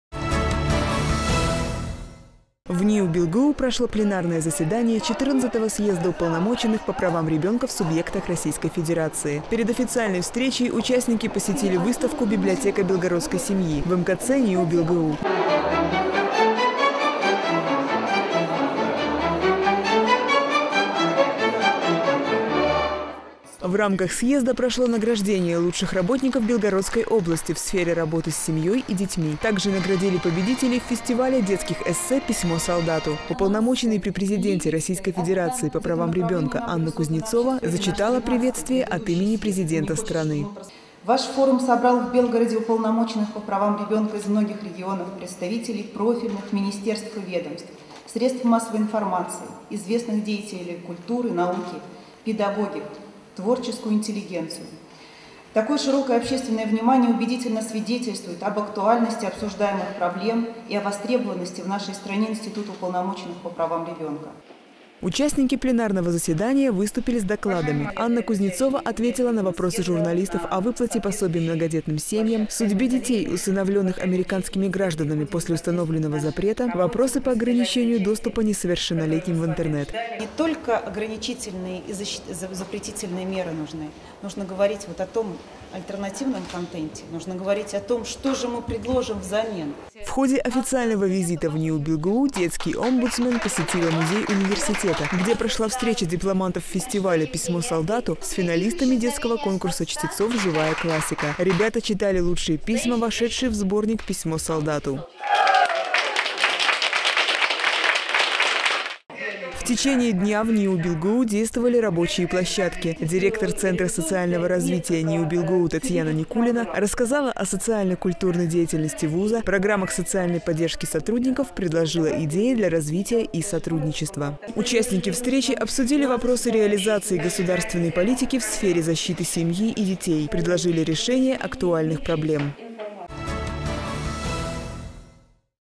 XIV съезд уполномоченных по правам ребёнка в НИУ «БелГУ»
В НИУ «БелГУ» прошёл 14 съезд уполномоченных по правам ребёнка в субъектах Российской Федерации. Темой форума стала «Защита права ребёнка жить и воспитываться в семье как приоритет государственной семейной политики».

Авторы: И. Лаврова, В. Глотов, Е. Николаичева, Ю. Виреева.